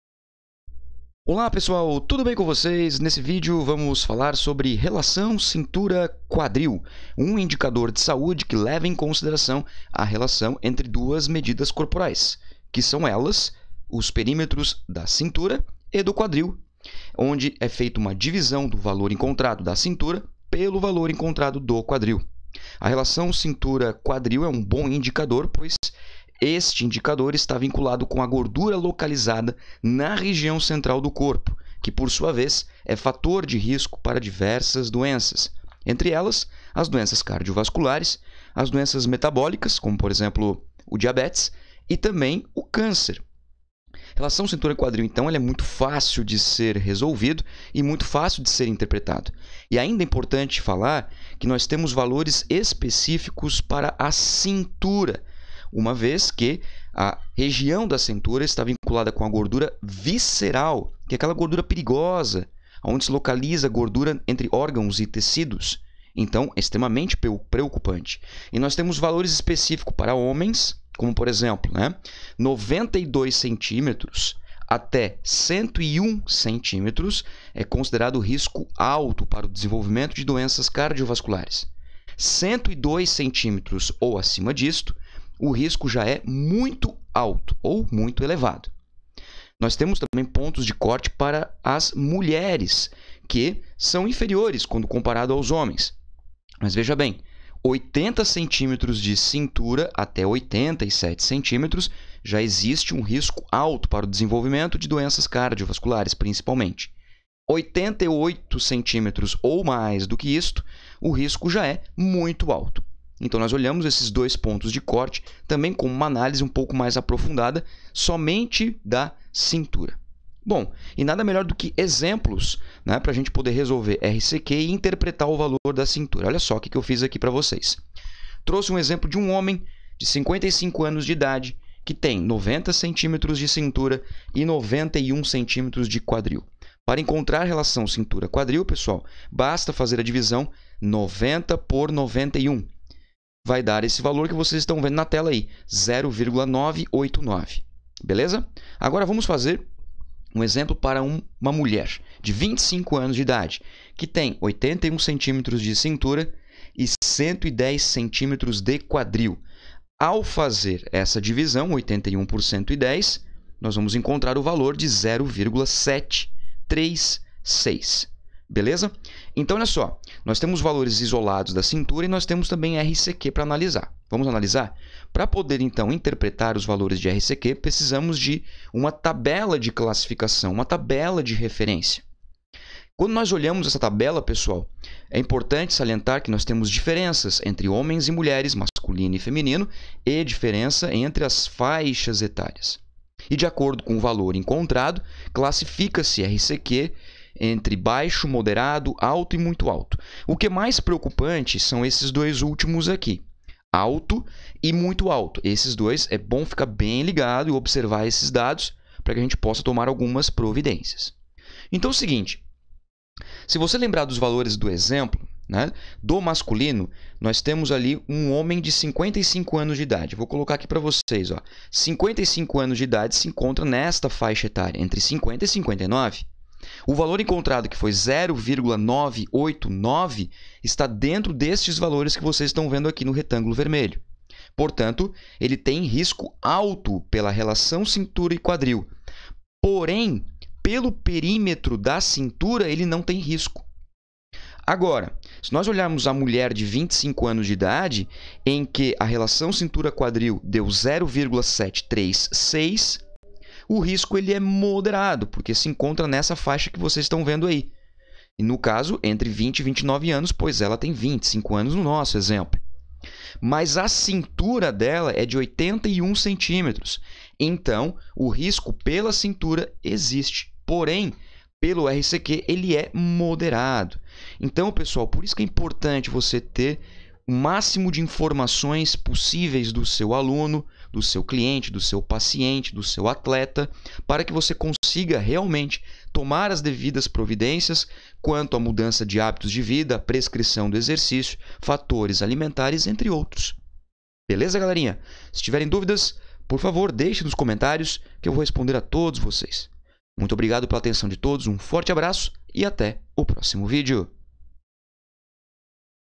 Relação Cintura-Quadril
A relação cintura-quadril é um indicador de fácil aplicação. Basta você dividir a medida da cintura pela medida do quadril, ambos os dados em centímetros.

Após isso, você deve olhar para tabela de classificação de acordo com a faixa etária e sexo para saber o grau de risco para desenvolvimento de doença cardiovascular.